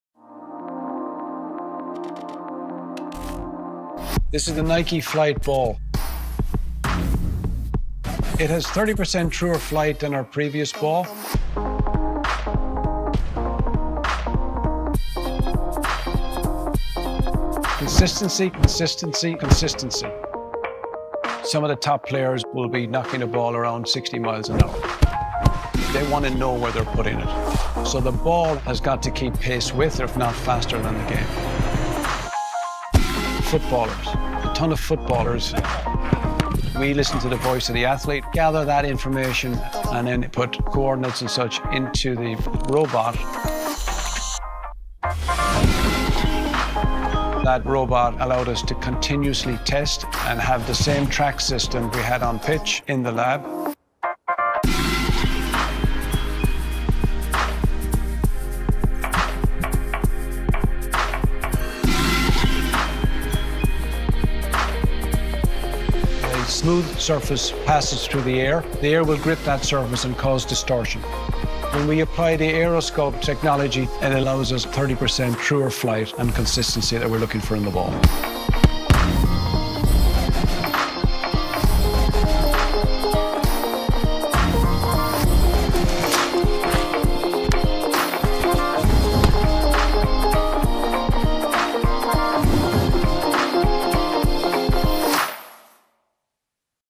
Nike Flight Ball | Behind the Design | Nike Football
1700 lab hours, 800 footballers, 68 iterations, and 1 robotic leg—All in pursuit of true flight. Learn the science behind the revolutionary new Nike Flight Ball featuring 30% truer flight than its Nike predecessor.